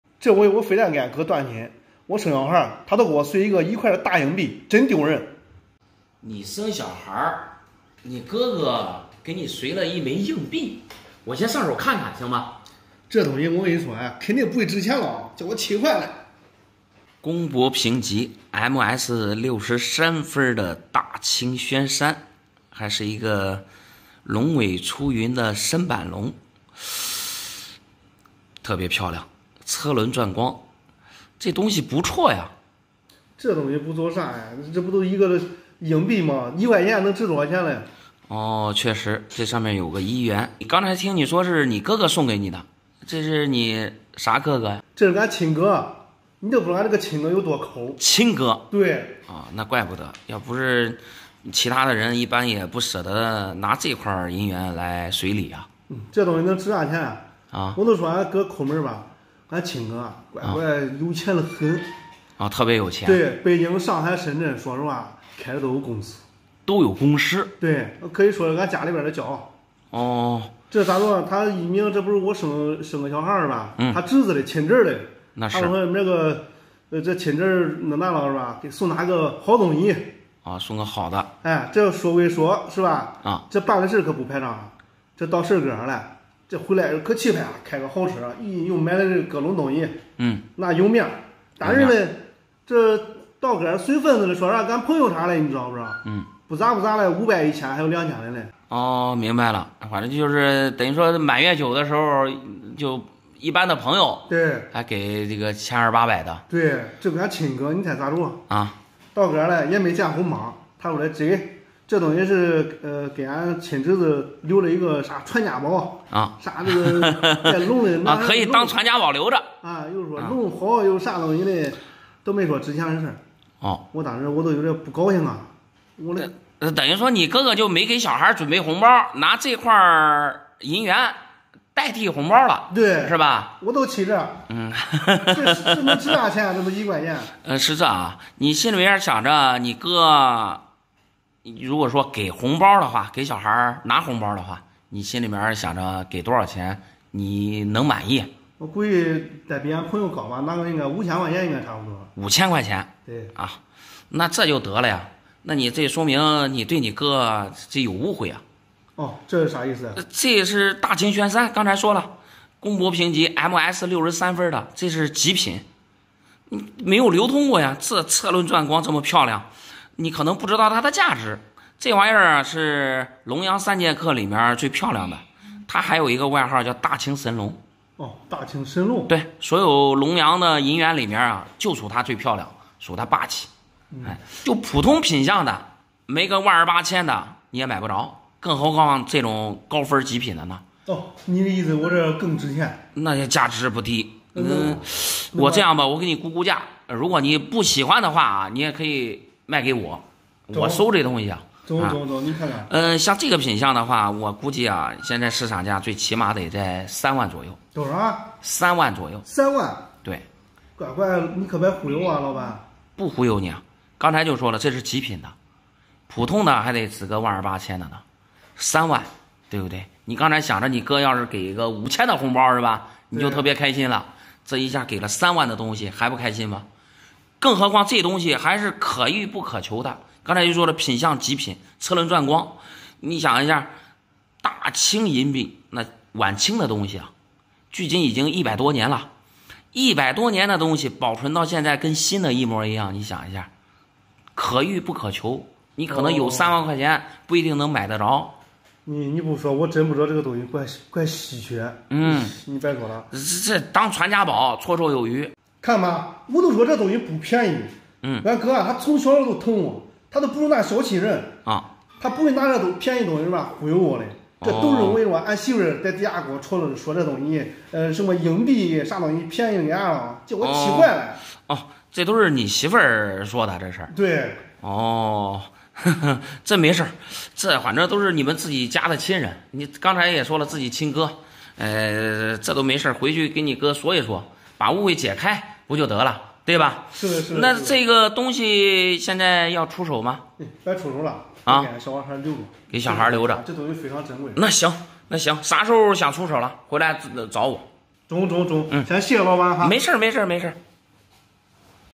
哥哥送极品宣三当见面礼，弟弟说是不值钱的硬币，估价居然值三万【小闫打铁镯】
请放心，本视频是一部原创拍摄的视频，没有任何不良行为。 哥哥送极品宣三当见面礼，弟弟说是不值钱的硬币，估价居然值三万